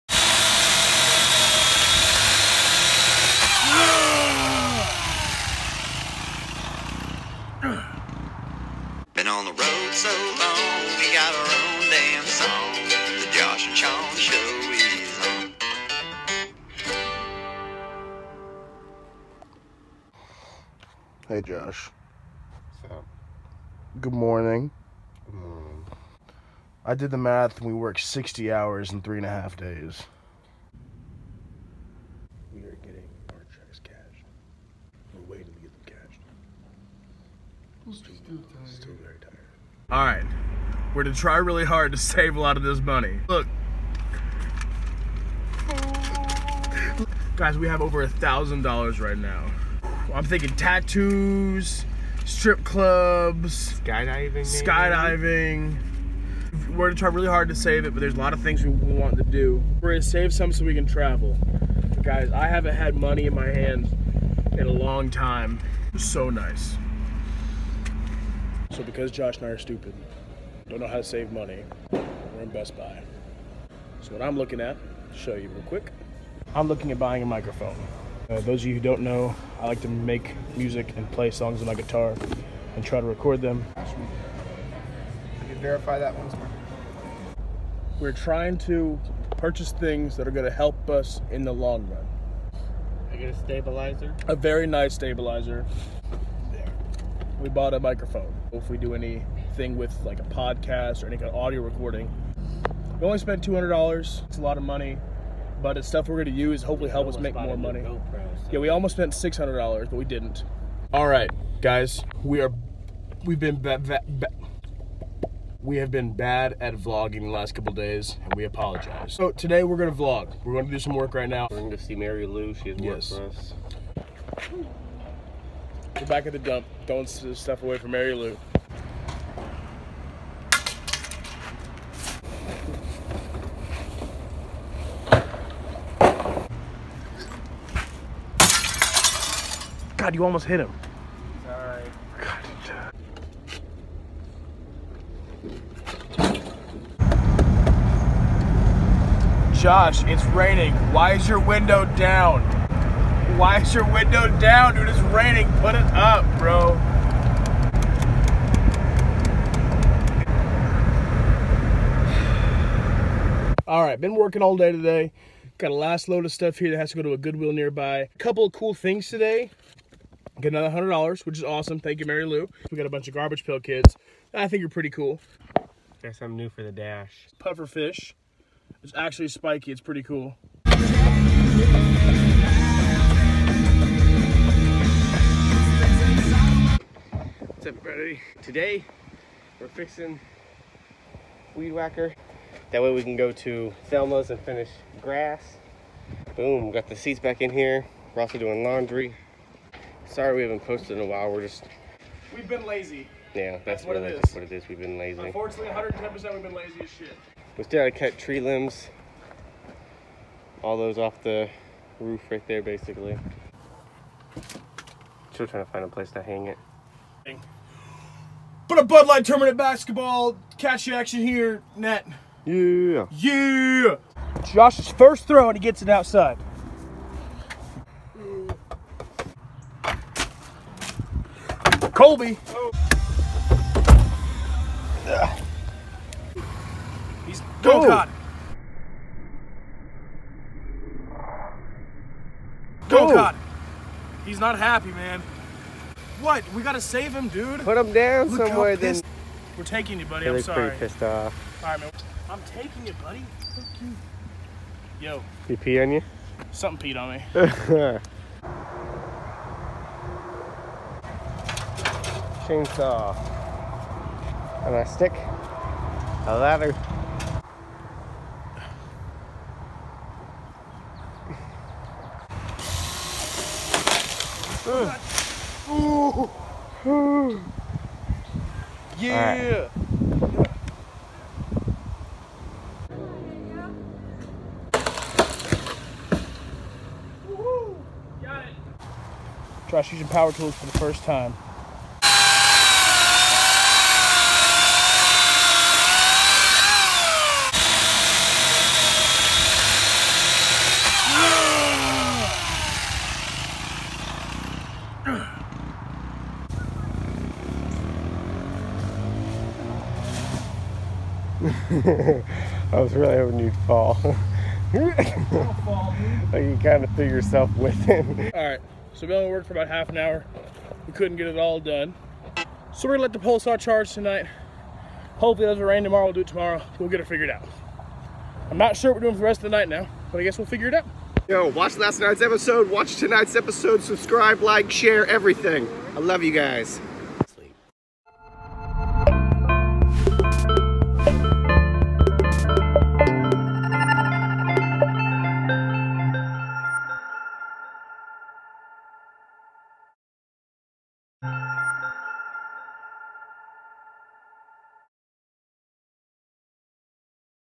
Days 165-169! Chainsaws, keyboards, and the dump!!!
It’s been a slow few days, but we’ve had a good time! Its been so much fun spending a little money, cutting down some branches, and going to the dump. Check it out and let us know what you think!
-Sean!

Yaya’s Beachside Bar - Dave’s bar!